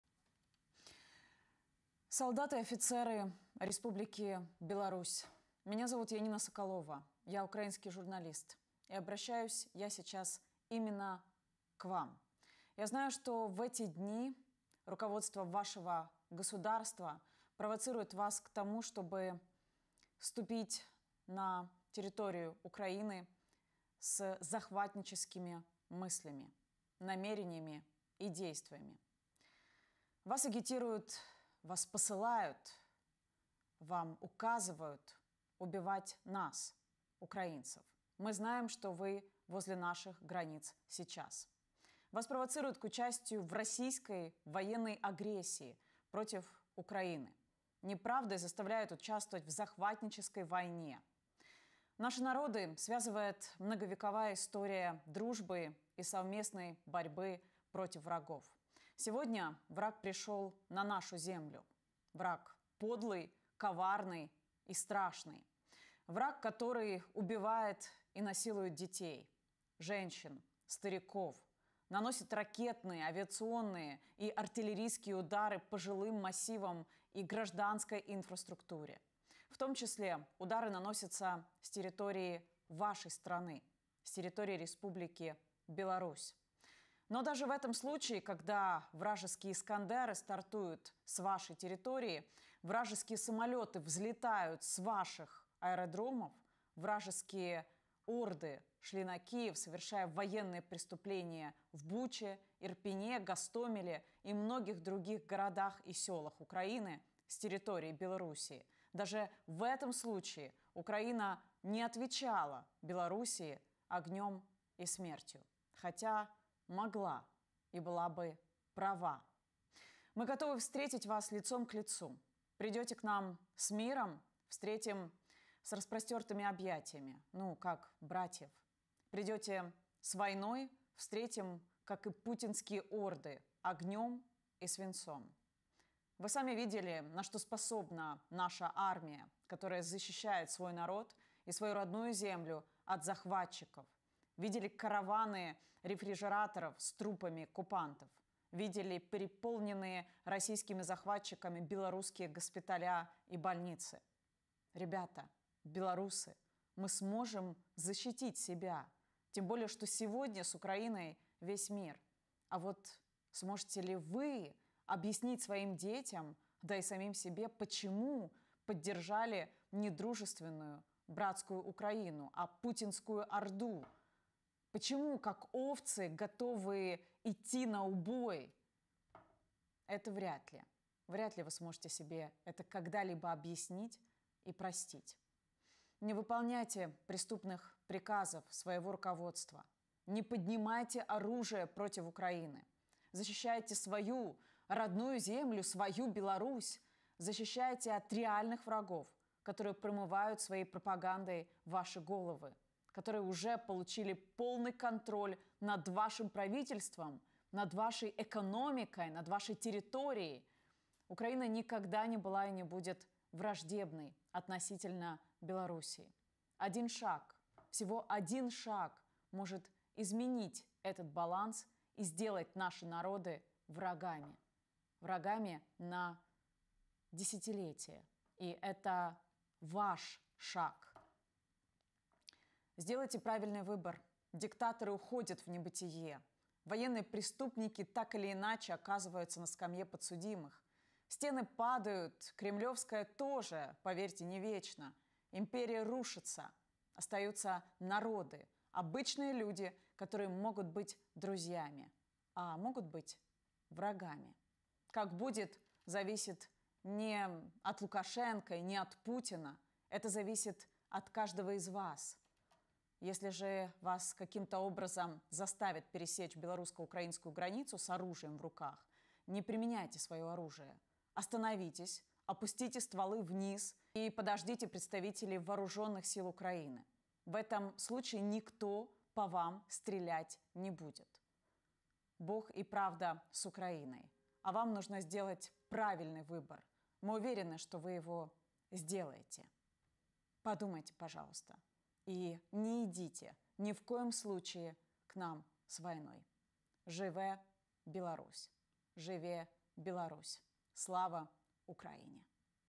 ⚡Білоруси біля кордону України. Термінове звернення Соколової
Термінове звернення Яніни Соколової до солдатів та офіцерів Білорусі.